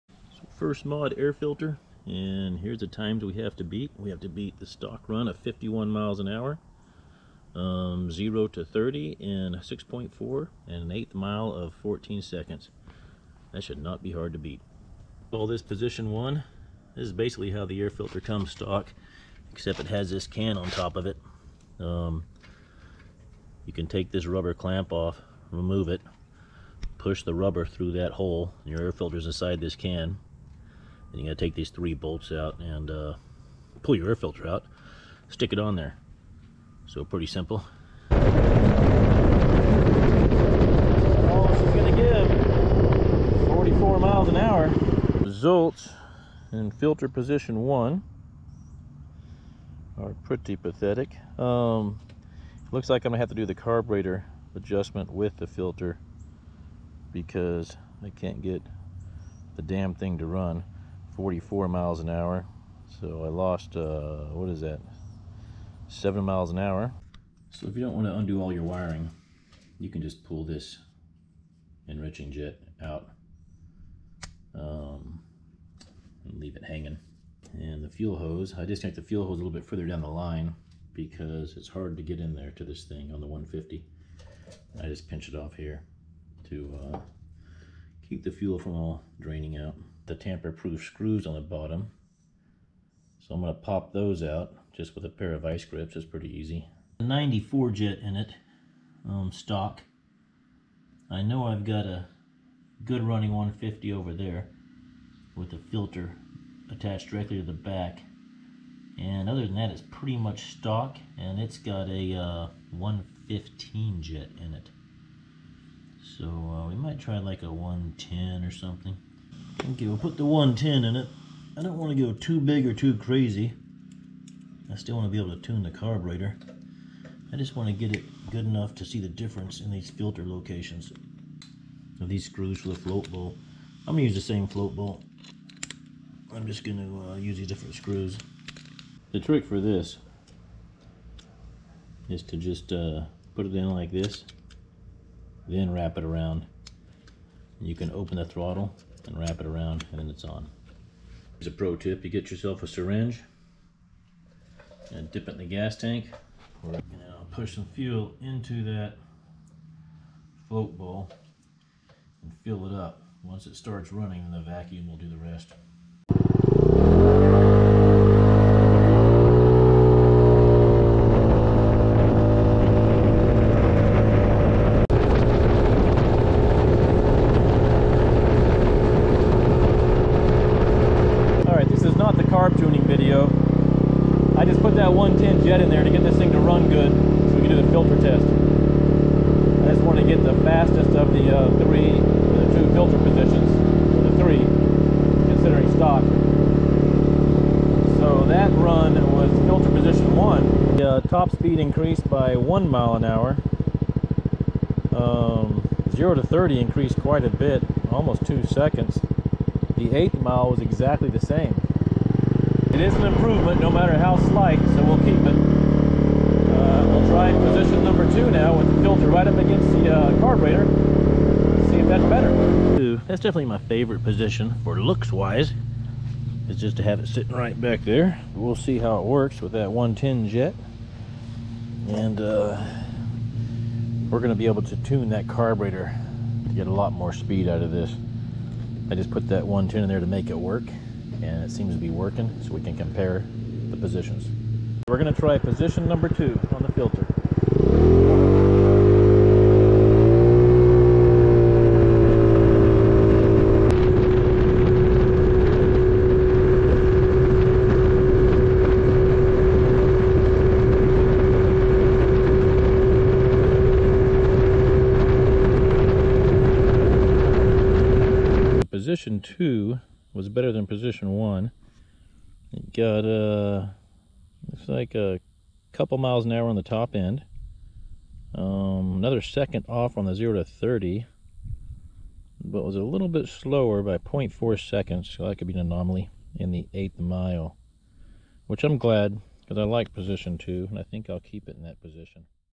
Maddog 150 Free Mod #1 (Air Filter) +4 mph.
icebear maddog 50 150, chuckus, ruckus
I will test each mod individually.  Mod to-do list:

FREE Mods
Air Filter
PAIR delete
842 Belt
Muffler insert
Cat delete
Adjust Valves
Tune Carb

MONEY MODS
NCY CDI
NCY direct coil
28mm smooth bore carb
A9 Cam
Electric Fuel Pump
171 BBK with 28.5mm Head

CVT MODS
Performance clutch
Larger variator
Roller weights
Final Drive Gear